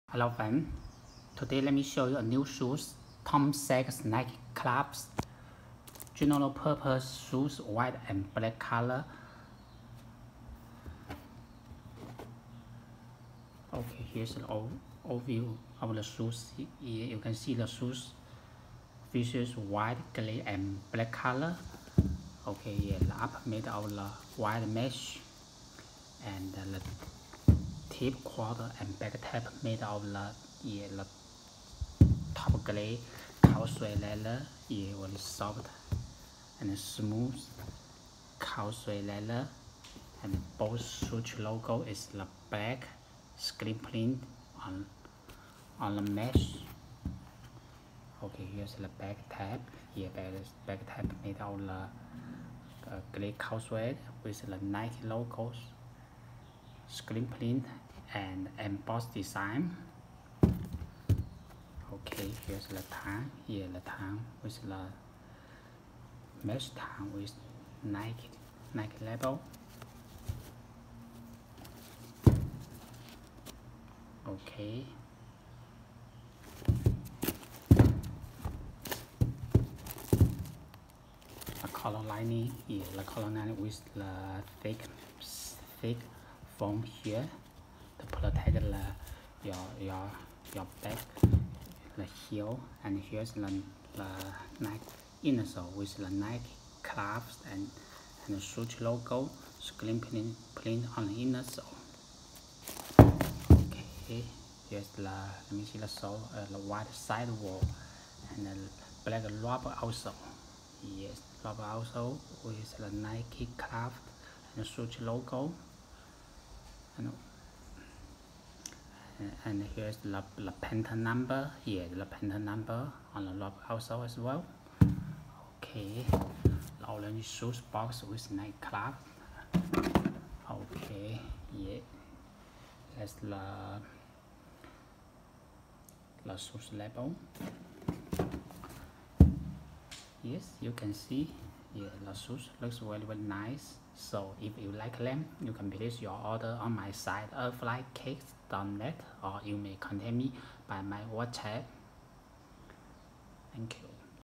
Where To Buy Tom Sachs x Nikecraft General Purpose Shoe White Black
Tom Sachs x Nikecraft General Purpose Shoe White Black

Hey friend today give you the 1st and detailed review on Tom Sachs x Nikecraft General Purpose Shoe White Black,pls click the below link to order on my

If you were a fan of Tom Sachs NikeCraft General Purpose Shoe, you’re in for a treat with a full collection of new designs for you to enjoy that include this white/black rendition.
Featuring a collection of three new colorways, this pair arrives after the Grey pair we took a look at a couple of days ago. Featuring the same simplistic vibes we all love, this pair opts for a classic colorway that anyone can get behind. Featuring a white mesh upper, the sneaker features white and alight grey being used throughout the leather and suede overlays. Black Swoosh logos are stamped across the sides while a classic Nike tongue label is accented by red tongue and heel tabs. A white rubber midsole and black outsole are added below to complete the design.
The next Tom Sachs x NikeCraft General Purpose Shoe collection does not have a release date set but a drop is expected to go down later this year via select Nike retailers and Nike.com. The retail price is set at $110. Enjoy detailed images of the pair below and make sure to follow JustFreshKicks for updates and more of the latest sneaker news from your favorite brands.